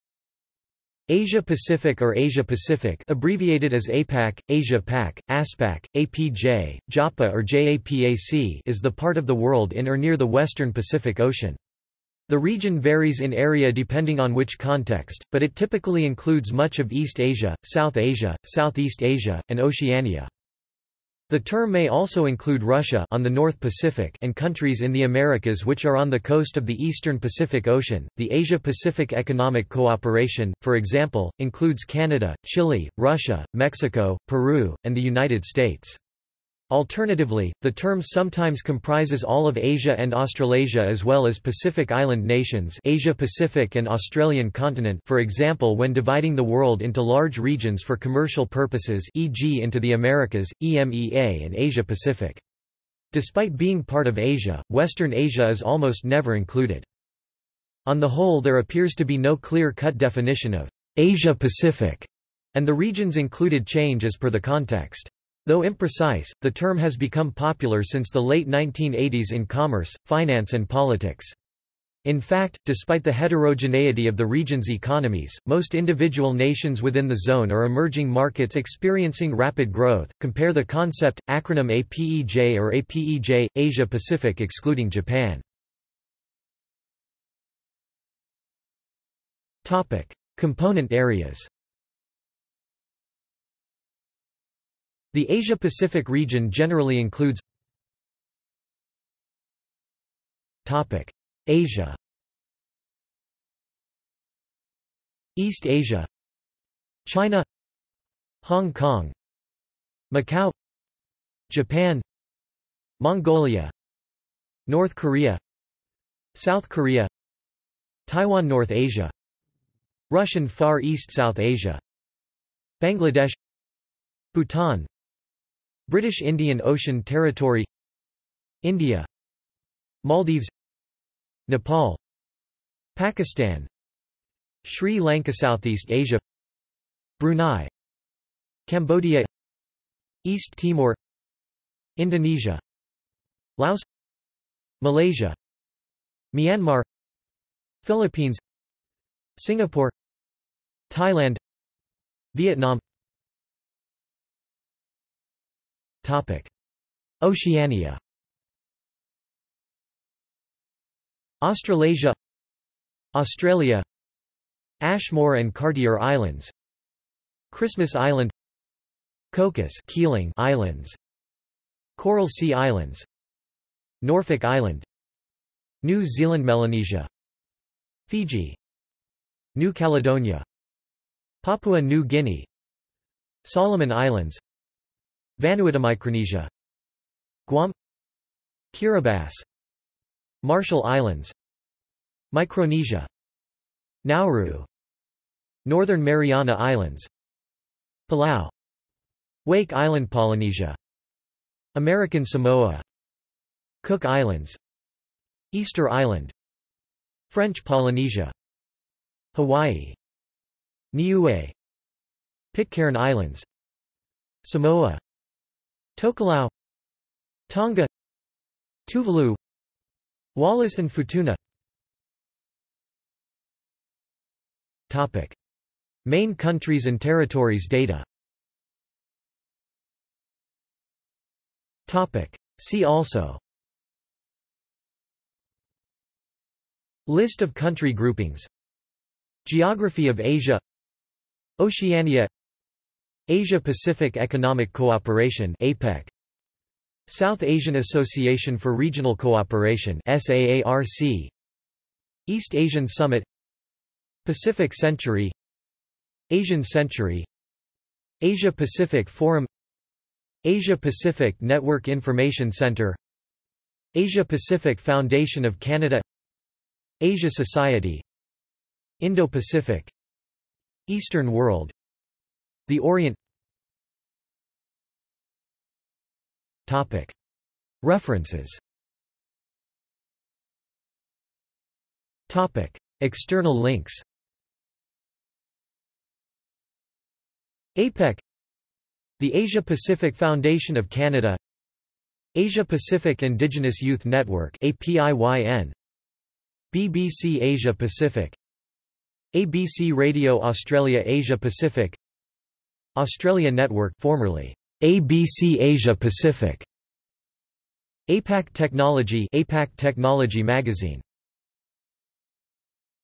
Asia-Pacific | Wikipedia audio article
This is an audio version of the Wikipedia Article:
Asia-Pacific

- Socrates

SUMMARY
Asia-Pacific or Asia Pacific (abbreviated as APAC, Asia-Pac, AsPac, APJ, JAPA or JAPAC) is the part of the world in or near the Western Pacific Ocean. The region varies in area depending on which context, but it typically includes much of East Asia, South Asia, Southeast Asia, and Oceania.
The term may also include Russia (on the North Pacific) and countries in the Americas which are on the coast of the Eastern Pacific Ocean; the Asia-Pacific Economic Cooperation, for example, includes Canada, Chile, Russia, Mexico, Peru, and the United States. Alternatively, the term sometimes comprises all of Asia and Australasia as well as Pacific island nations (Asia Pacific and Australian continent) - for example when dividing the world into large regions for commercial purposes (e.g. into the Americas, EMEA and Asia Pacific). Despite being part of Asia, Western Asia is almost never included.
On the whole there appears to be no clear cut definition of "Asia Pacific" and the regions included change as per the context. Though imprecise, the term has become popular since the late 1980s in commerce, finance and politics. In fact, despite the  heterogeneity of the regions' economies, most individual nations within the zone are emerging markets experiencing rapid growth. (Compare the concept/acronym APEJ or APeJ - Asia-Pacific excluding Japan.)